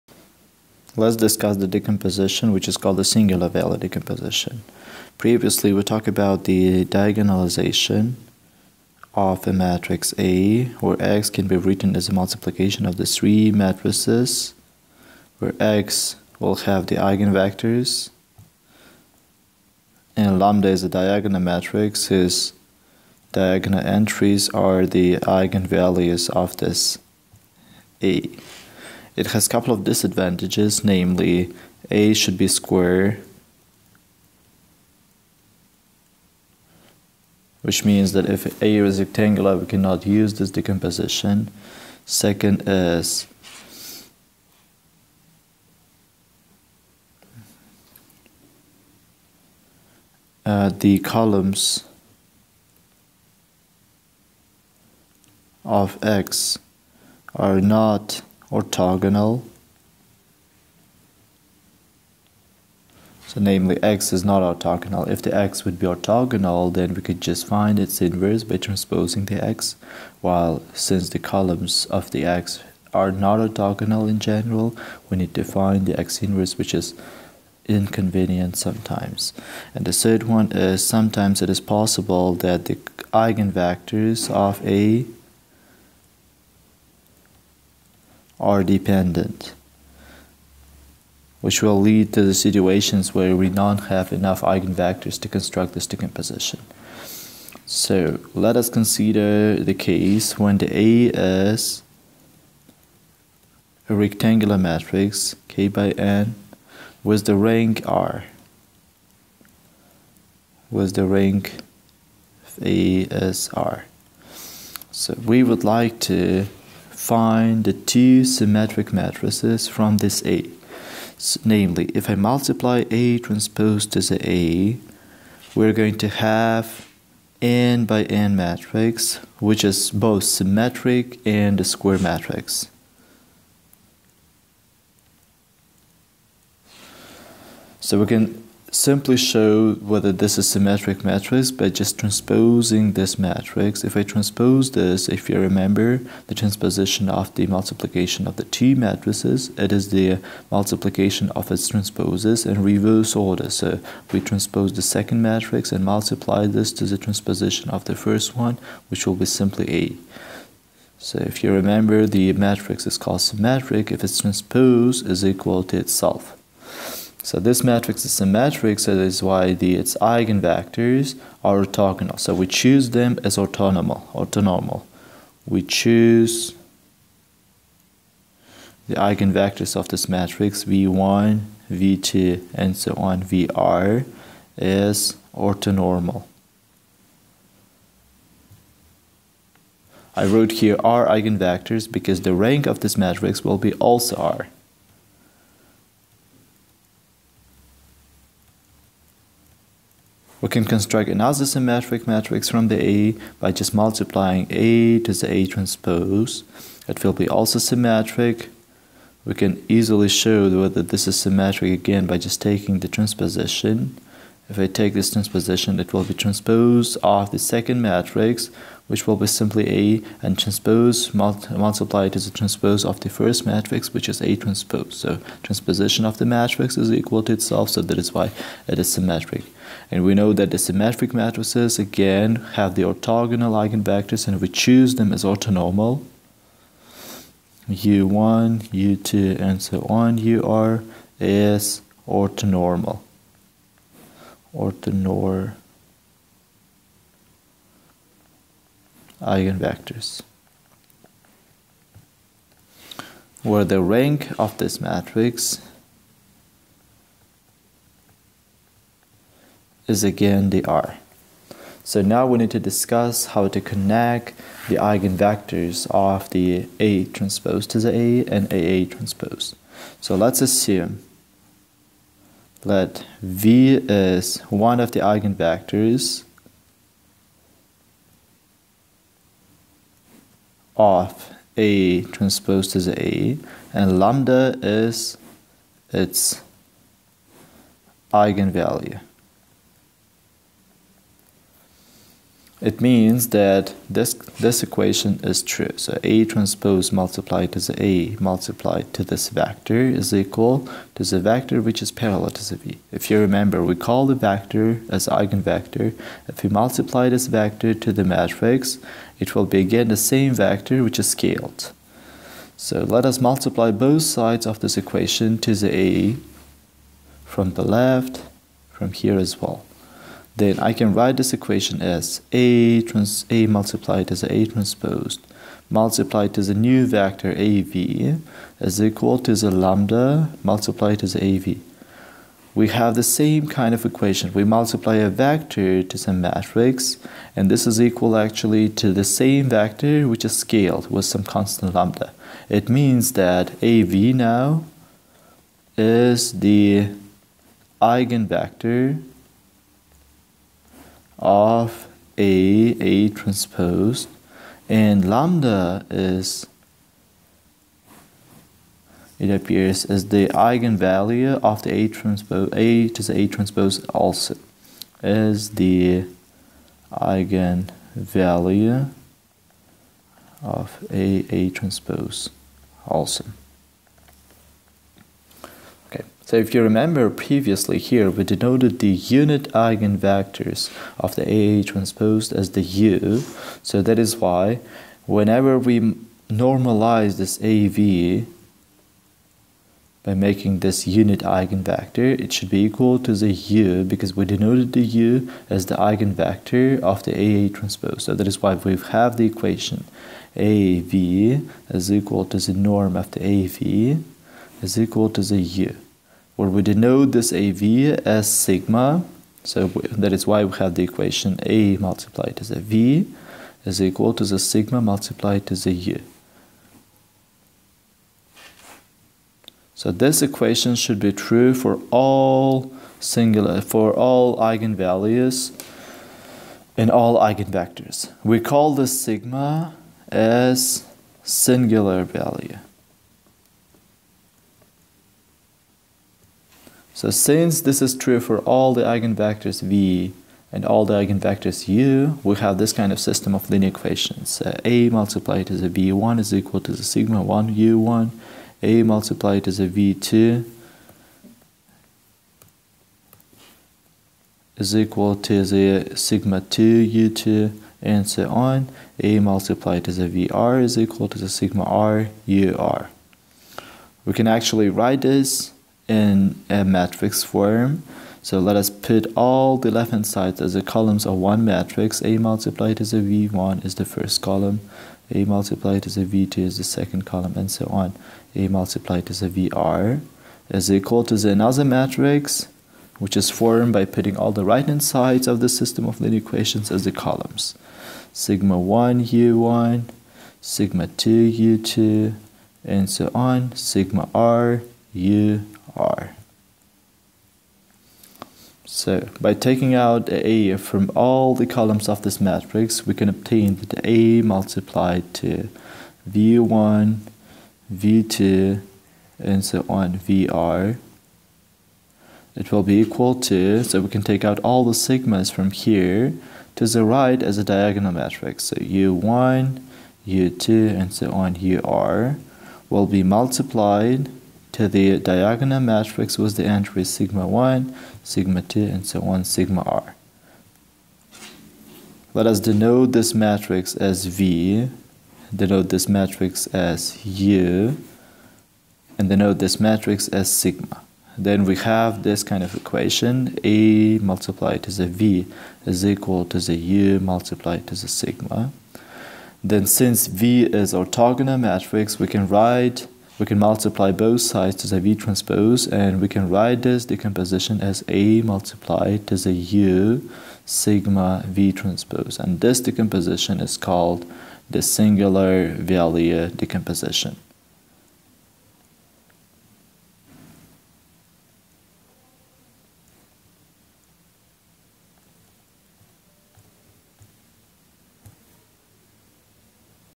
SVD (Singular Value Decomposition): Complete Explanation
This is an introduction to singular value decomposition. This video explains the basic idea of construction of the singular value decomposition for rectangular matrices.